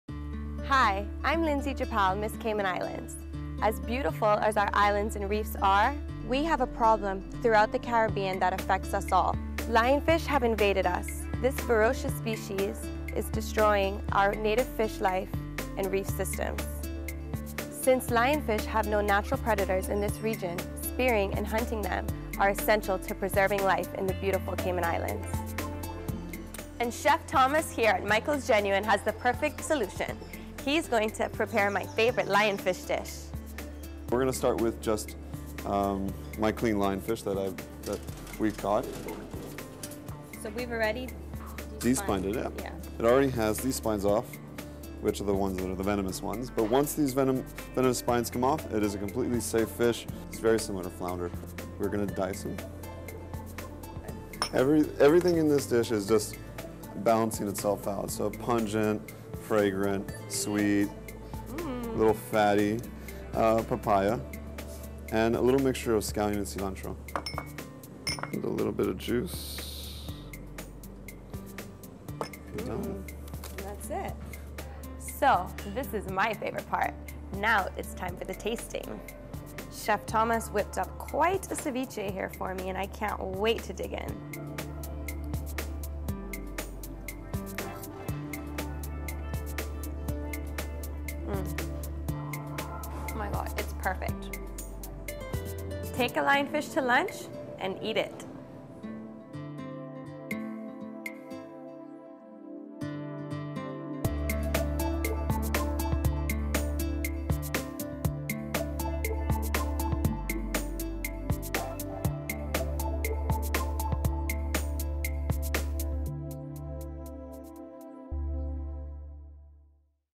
Lionfish University PSA Lindsay Japal
Ms. Cayman Lindsay Japal marries marine conservation with Michael's Genuine culinary expertise to show you a delicious new way to help with our islands Lionfish problem.